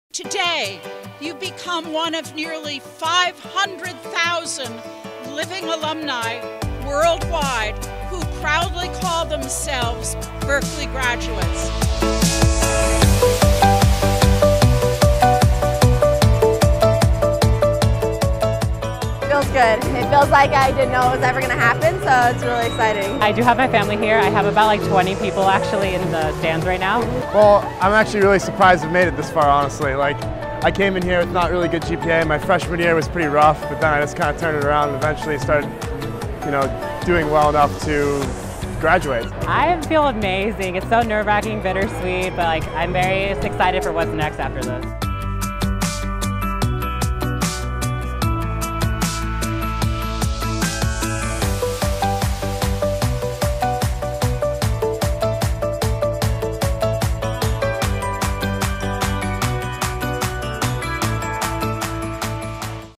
Spring Commencement 2018
CalTV speaks to UC Berkeley's newest graduates about their latest accomplishment—graduating from the number 1 public university in the world. Congratulations Class of 2018!